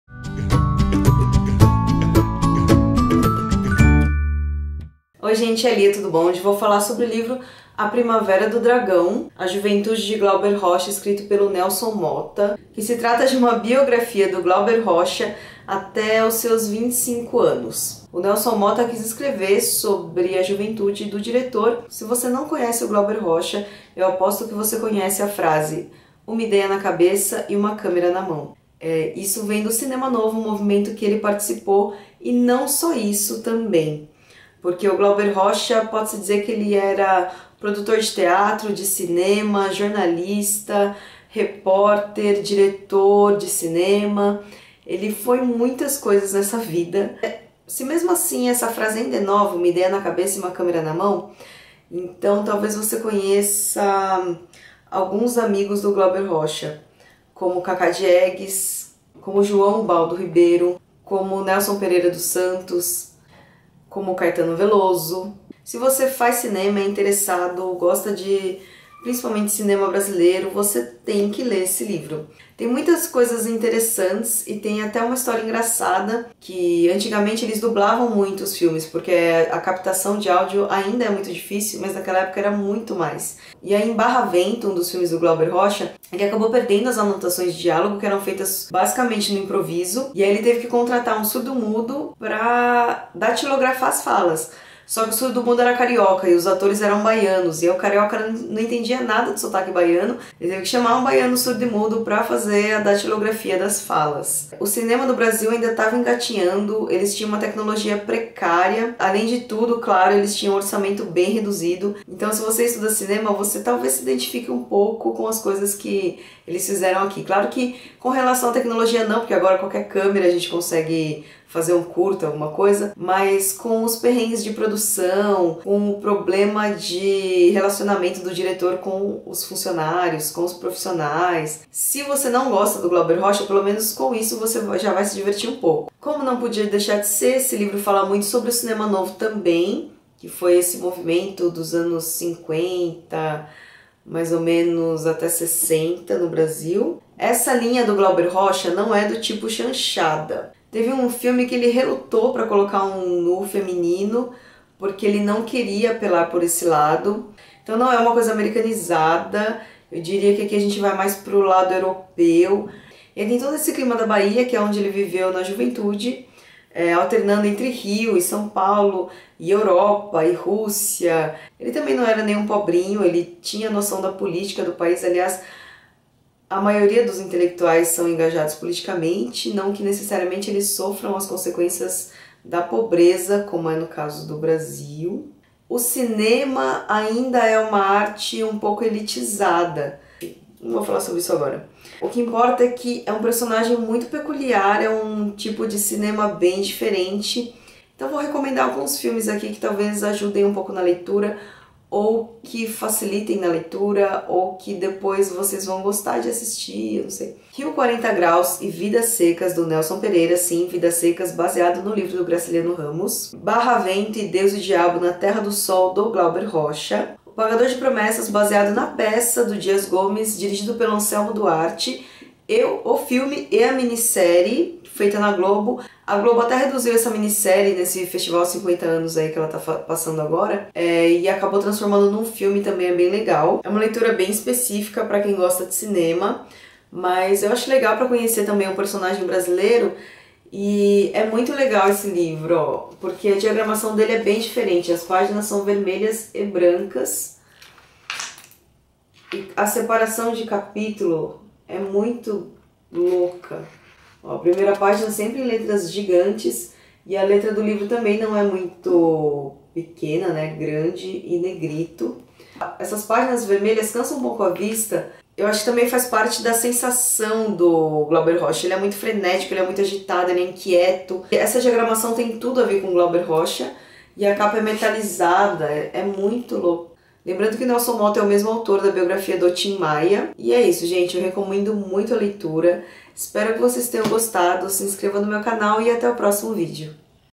RESENHA - A primavera do dragão, Nelson Motta
Mais um vídeo perdido, que foi encontrado!!!

Título: A primavera do dragão, a juventude de Glauber Rocha
Autor: Nelson Motta
Editora: objetiva
Ano: 2011
Páginas: 368
Edição: 1